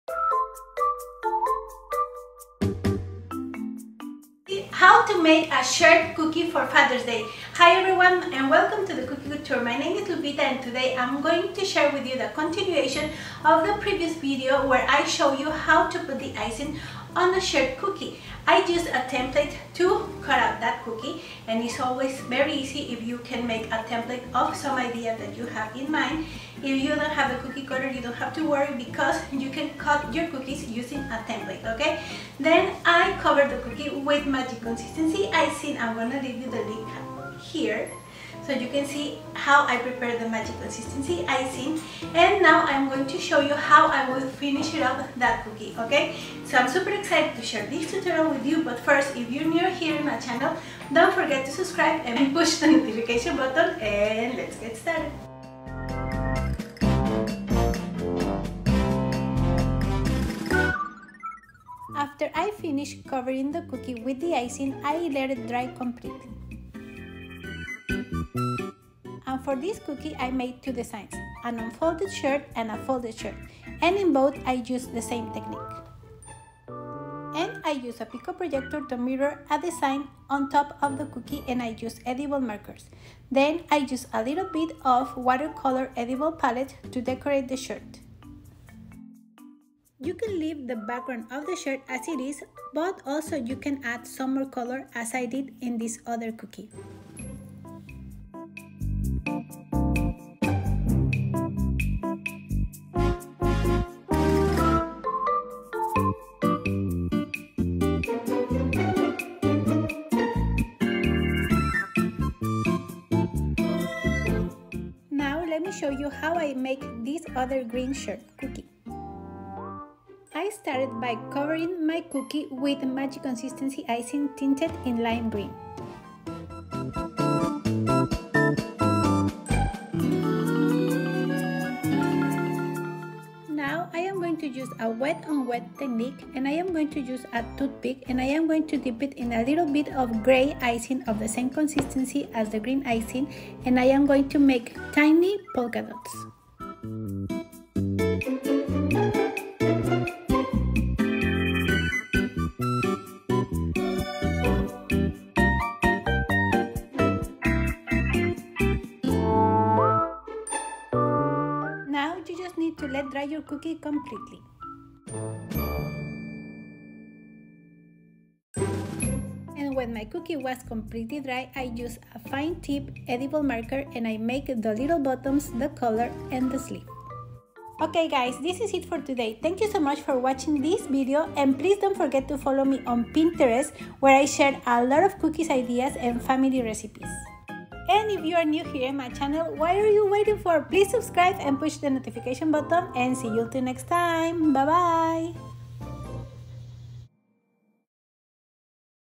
Father's day cookie decorating ideas - Shirt Cookie
In this video you will see different  ways to decorate a shirt cookie for Father's Day/Father's Day cookie decorating ideas.

Akaso Pico Projector
Sweet sticks edible watercolor palette

How to make a Hawaiian shirt cookie decorating tutorials

Amazon
Kitchen Aid